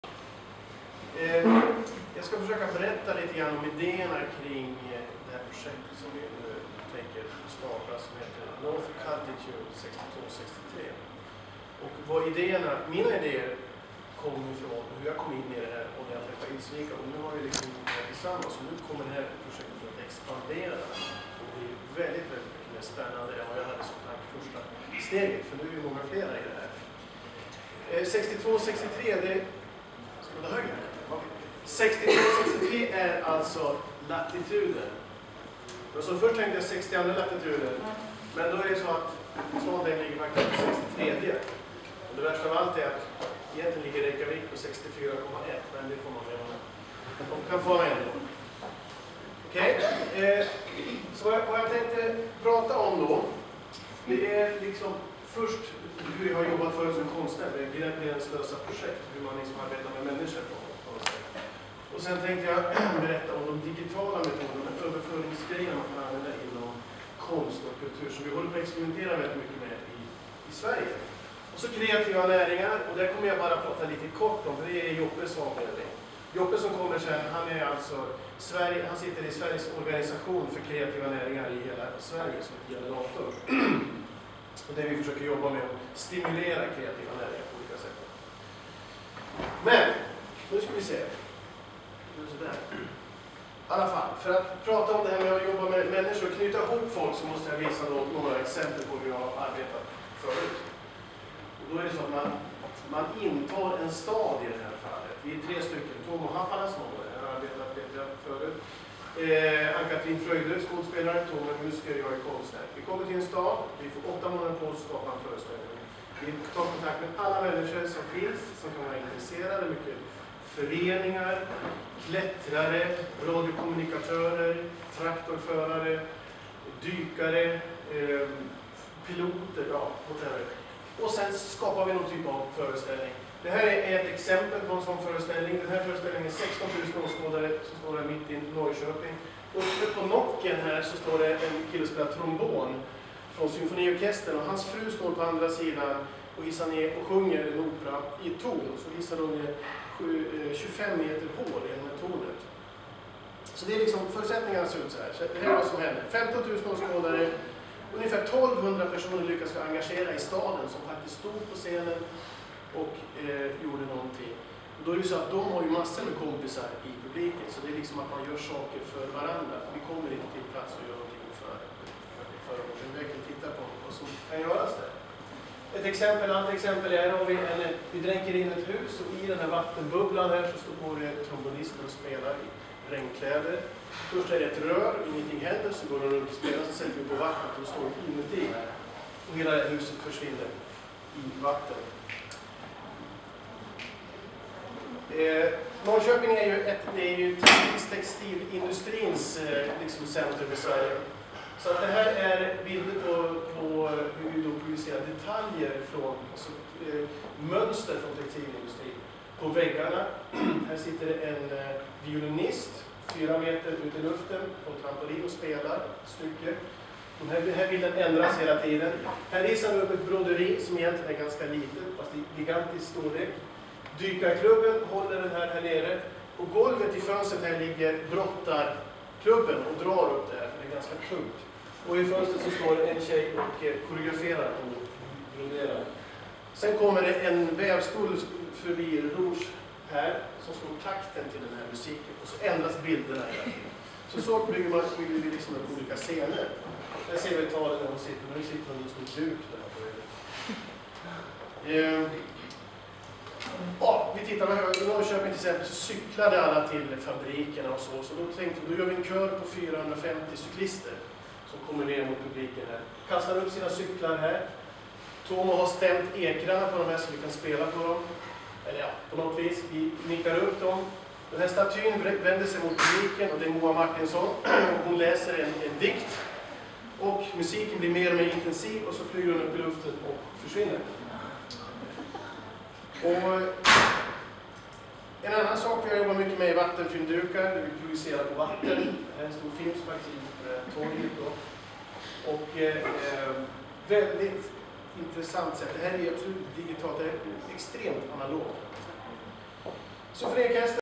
62' 63' North Cultitude 2012 - Presentasjon del 1
Kunstneren Andreas Brännlund presenterer 62' 63' North Cultitude på Atelier Ilsvika, Trondheim (Norge). Mars 2012

About North Cultitude (english):
North Cultitude 6263´s vision is to increase cooperation in the latitudes 62 and 63 with art projects / cultural projects. The fact is that the Internet is connecting artists and designers to create interactive joint projects in the latitudes 62 and 63. The aim is that within 10 years artists have been linked together and created events and biennials, in all latitudes around the world.